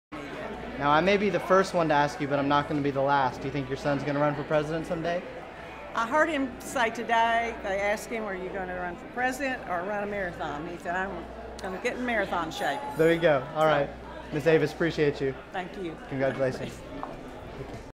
Avis Cotton on whether U.S. Sen.-elect Tom Cotton will run for presdient
Arkansas Times David Ramsey asks Avis Cotton whether her son, Tom Cotton, will run for president.